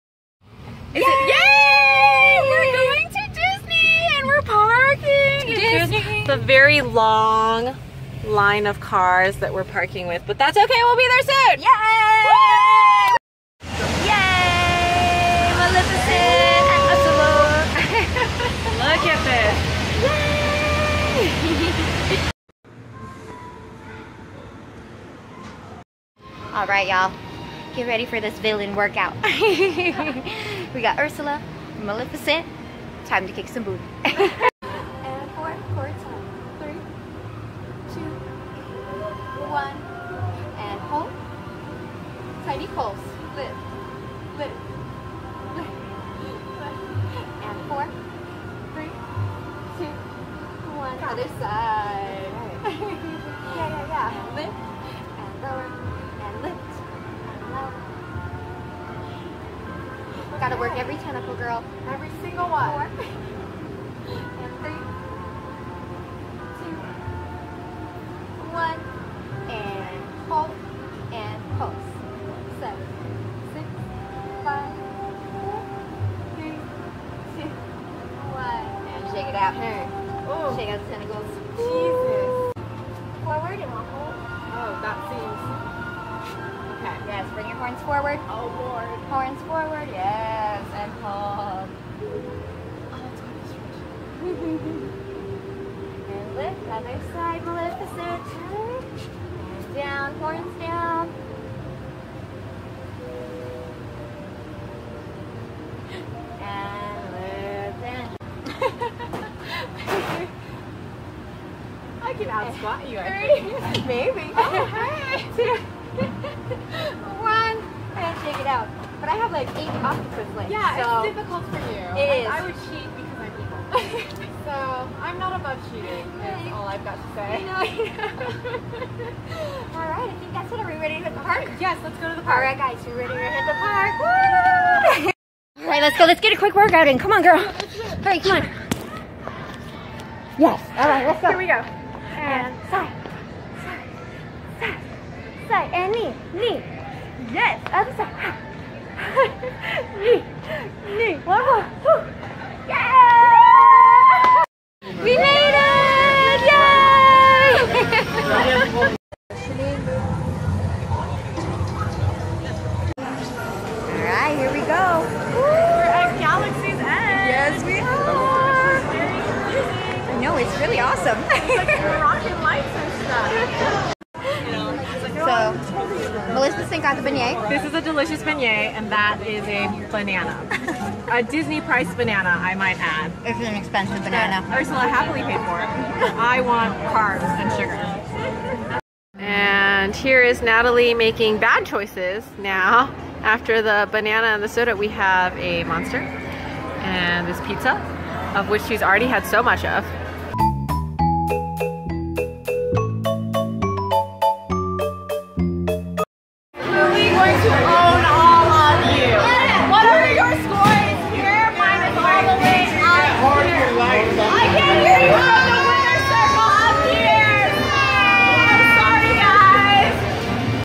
Disneyland Vlog- Sporty Villains
Judy and I had a spectacular day working out and riding rides as Sporty Villains at Disneyland! Judy was Maleficent and I was Ursula. There's a longer workout section at the beginning that I didn't want to cut because you can hear all of Judy/Maleficent's trash-talking LOL I hope you enjoy our goofy adventures :P